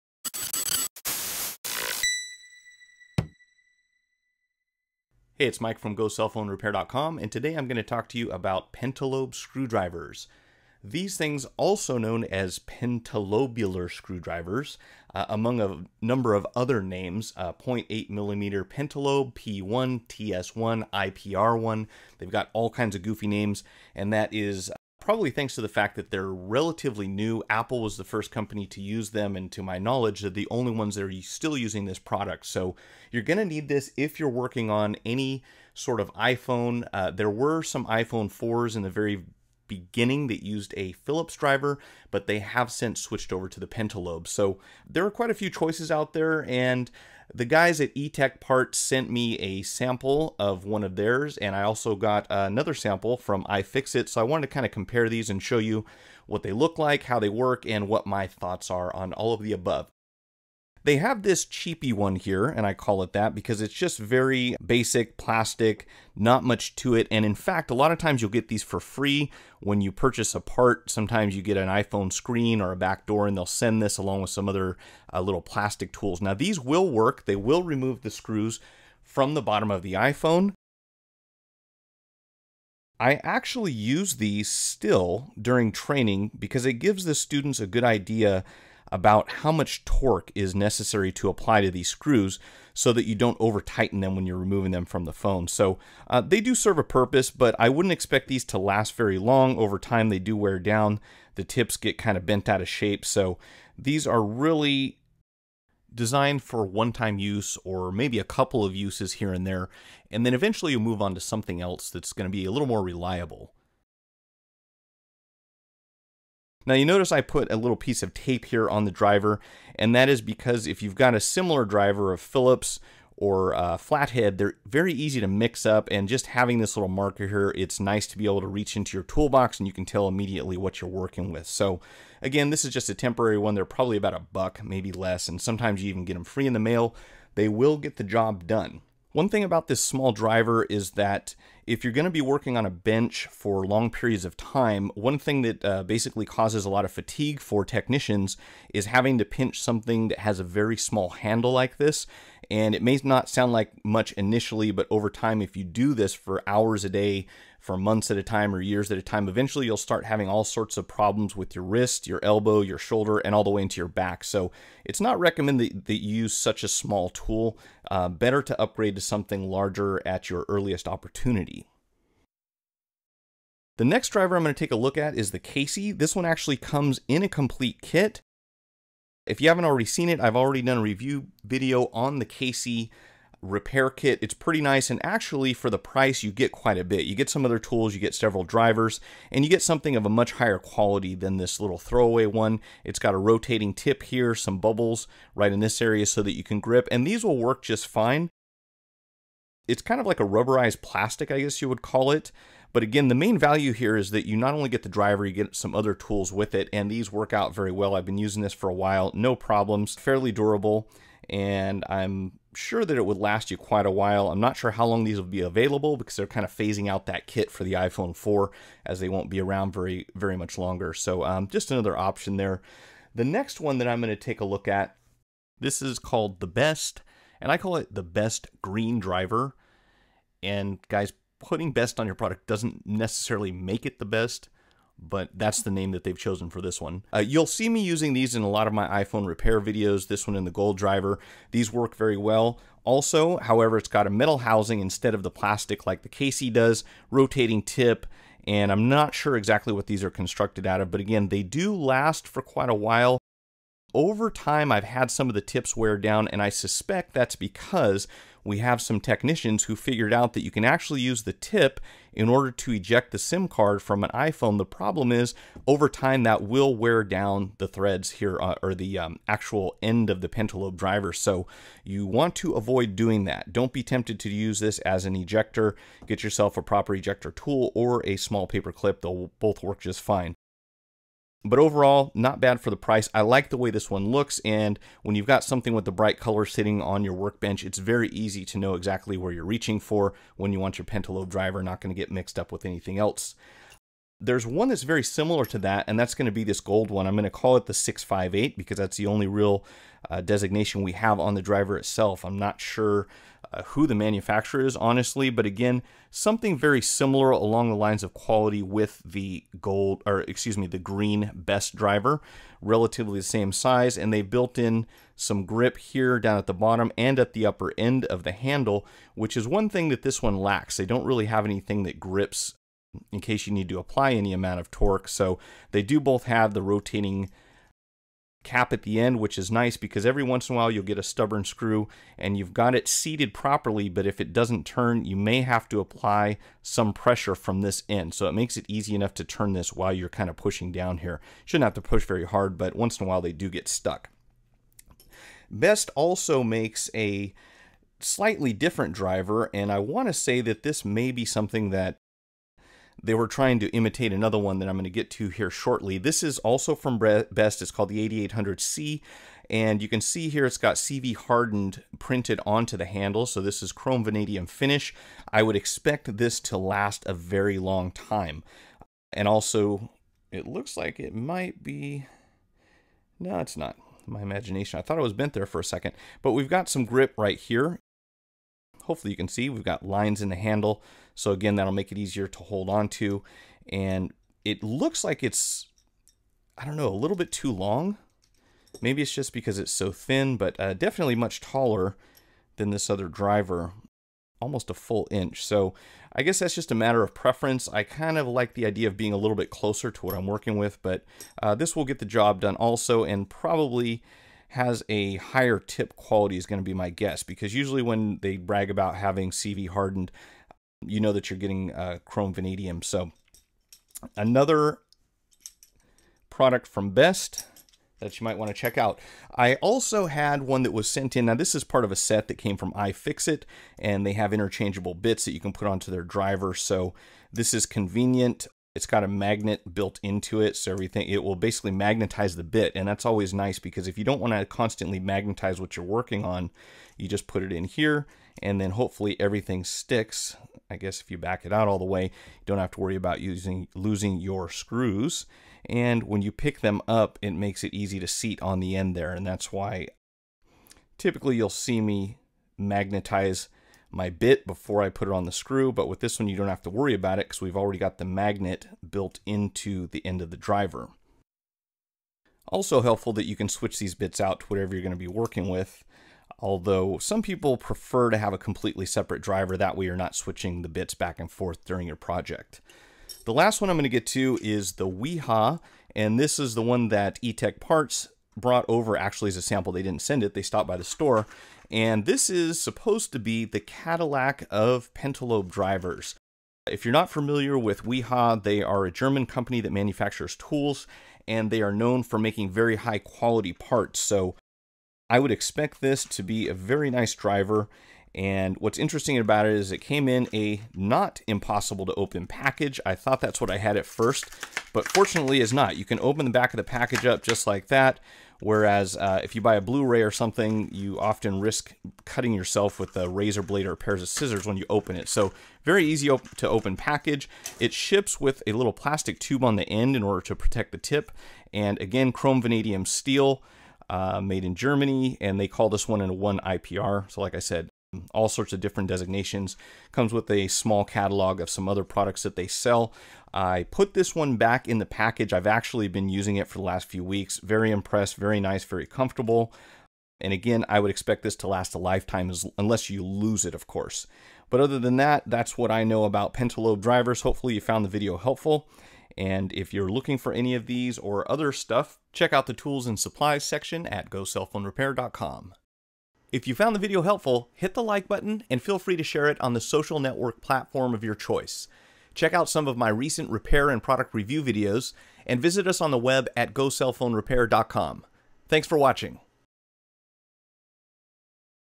What is a Pentalobe Screwdriver?
In this video we take a look at several different Pentalobe screwdrivers that are used for opening Apple products.